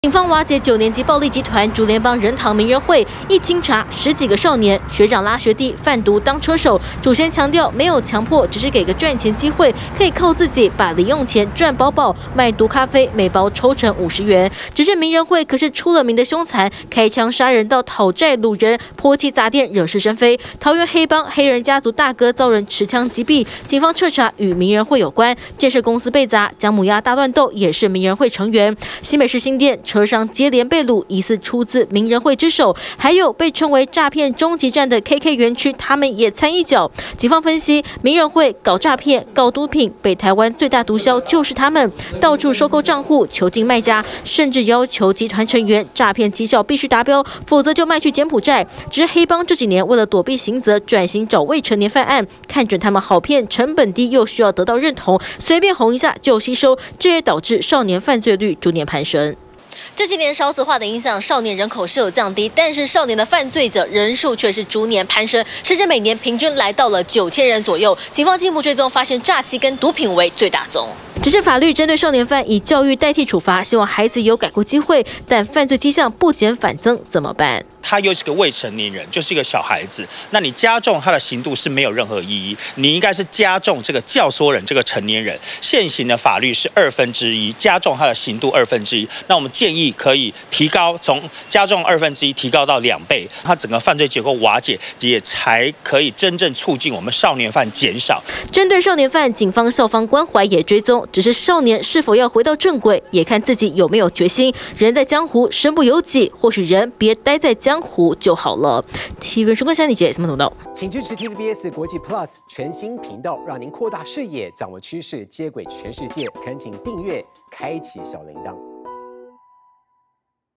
瓦解9年級暴力集團!學長拉學弟販毒 律師:應加重教唆刑度｜TVBS新聞
持續追蹤這個暴力集團！
警方清查成員，有不少都是青少年，學弟跟著學長販毒、當車手，以為能靠自己賺零用錢，但越走越偏，這幾年黑幫轉型，吸收年輕人當成員，看準他們好騙、成本低，刑度也比較輕，又渴望被人認同，對此就有律師建議，要加重教唆者刑度，才能遏止「少年犯」攀升。